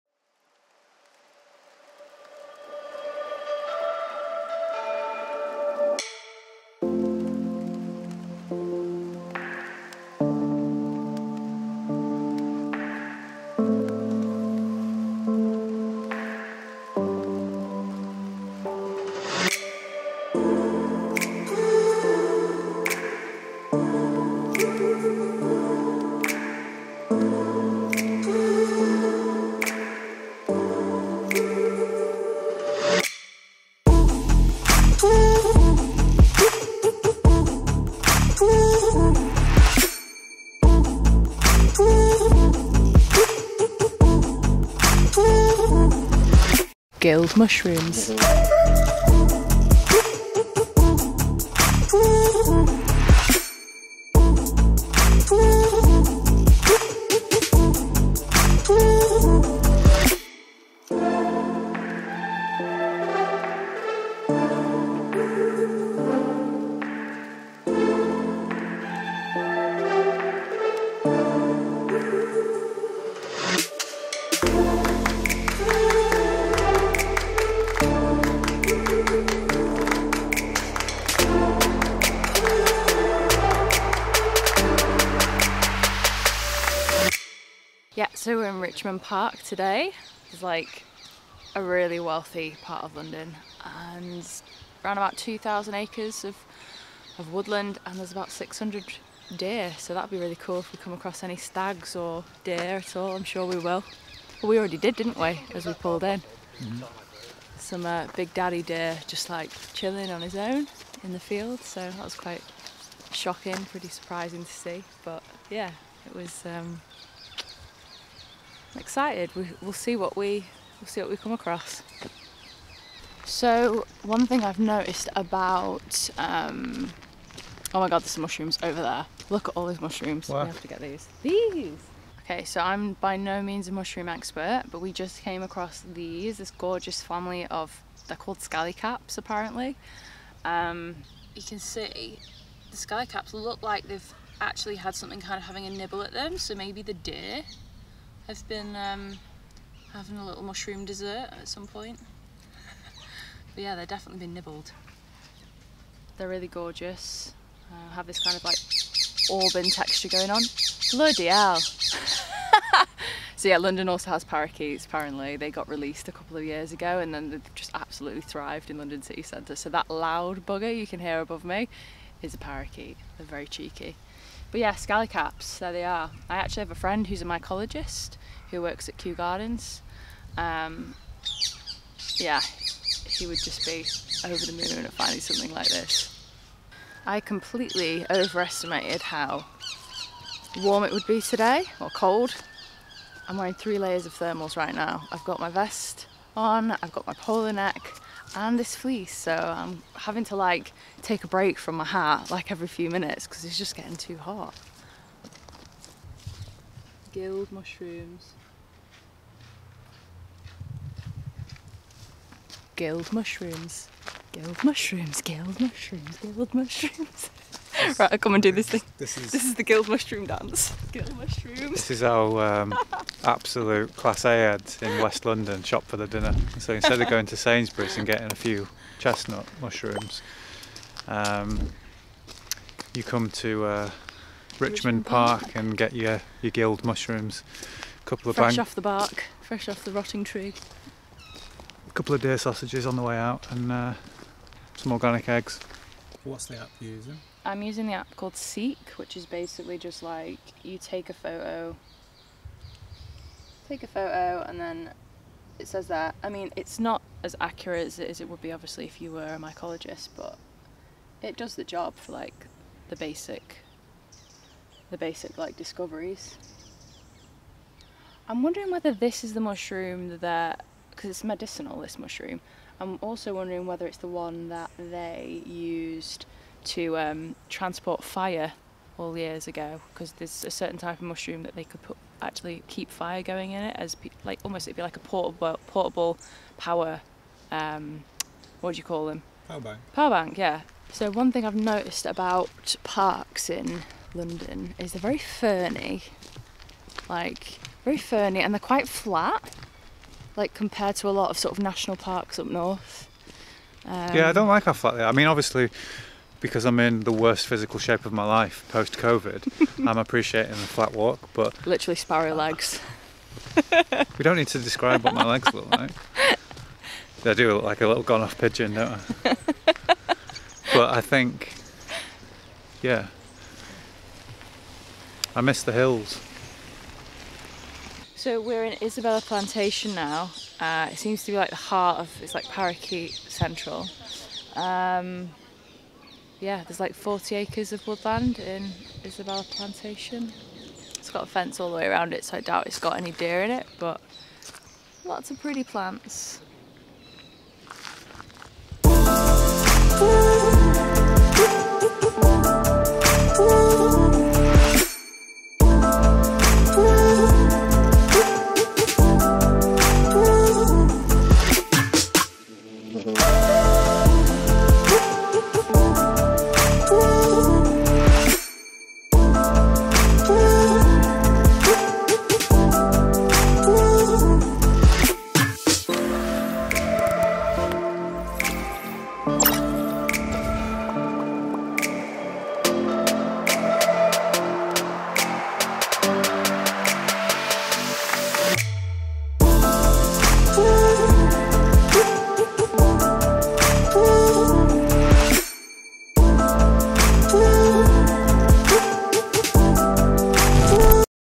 A Winter Walk in Richmond Park.
If you like majestic deer, cheeky parakeets and magic mushrooms… you’ll love this video. 🦌🦜🍄

Lockdown was rough for everyone. So, when rules were lifted, we rushed out to explore one of the greenest places in London: Richmond Park.

It is nestled in the London Borough of Richmond upon Thames and was created specially as a deer park in the 17th century by Charles the 1st.

As you’ll see in the video, it’s the largest of London's Royal Parks with multiple ponds, snakelike trails, a plantation, playgrounds and plenty of woodland. Safe to say, Richmond Park proves to be a crucial conservation gem both nationally and internationally for our precious wildlife.

Gear note: I was trying out a new Variable ND filter from URTH/Gobe on my Lumix G9. The filter is awesome and made so many shots possible in the bright sunshine. However, like a whopper I left my camera on auto white balance so some of the shots look a bit pink as I had to try and correct it all. Lesson learnt.

🔶Gear I use